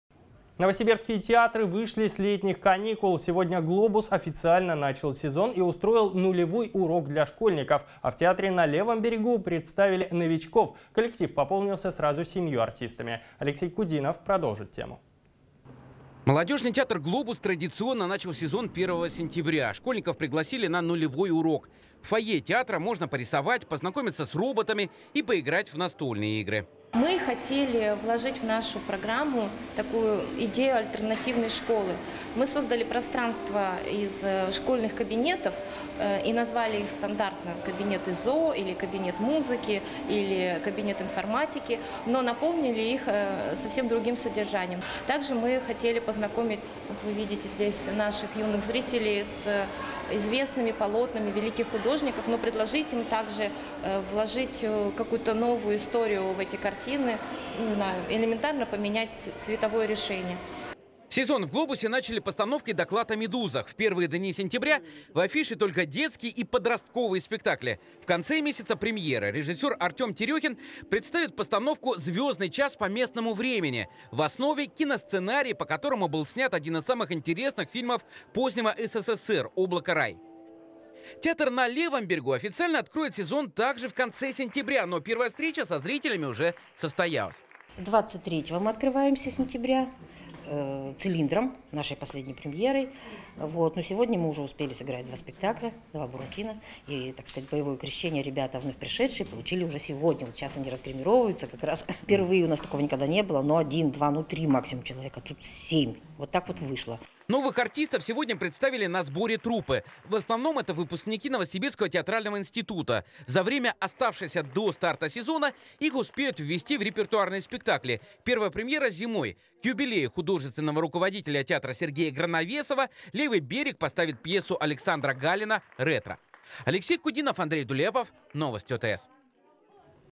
Новосибирские театры вышли с летних каникул
"Глобус" официально начал сезон и устроил "нулевой урок" для школьников. А в театре "На левом берегу" представили новичков. Коллектив пополнился сразу семью артистами.